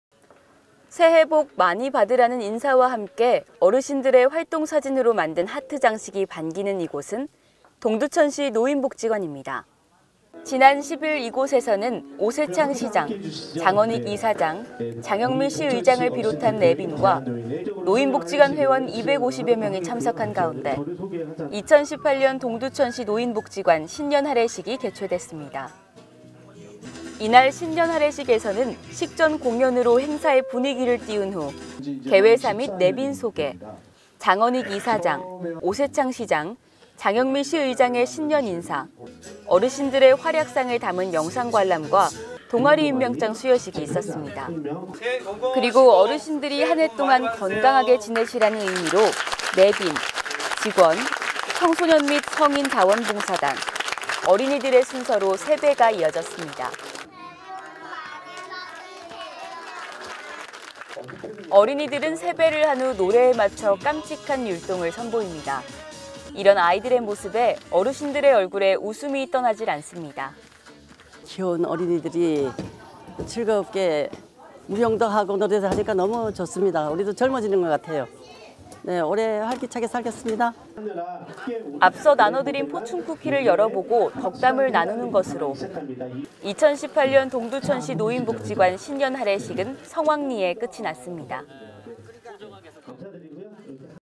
2018년 1월 10일 노인복지관 신년하례식
새해 복 많이 받으라는 인사와 함께 어르신들의 활동사진으로 만든 하트 장식이 반기는 이곳은 동두천시 노인복지관입니다

지난 10일 이곳에서는 오세창 시장, 장헌익 이사장, 장영미 시의장을 비롯한  내빈과 노인복지관 회원 250여명이 참석한 가운데 2018년 동두천시노인복지관 신년하례식이 개최됐습니다.

이날 신년하례식에서는 식전공연으로 행사의 분위기를 띄운 후 개회사 및 내빈소개, 장헌익 이사장, 오세창 시장, 장영미 시의장의 신년인사 어르신들의 활약상을 담은 영상 관람과 동아리 임명장 수여식이 있었습니다.

그리고 어르신들이 한 해동안 건강하게 지내시라는 의미로 내빈, 직원, 청소년 및 성인 자원봉사단, 어린이들의 순서로 세배가 이어졌습니다.

어린이들은 세배를 한 후, 노래에 맞춰 깜찍한 율동을 선보였습니다. 이런 아이들의 모습에 어르신들의 얼굴에 웃음이 떠나질 않았습니다. 앞서 나눠 드린 포춘쿠키를 열어보고 덕담을 나누는 것으로 2018년 동두천시 노인복지관 신년하례식은 성황리에 끝이 났습니다.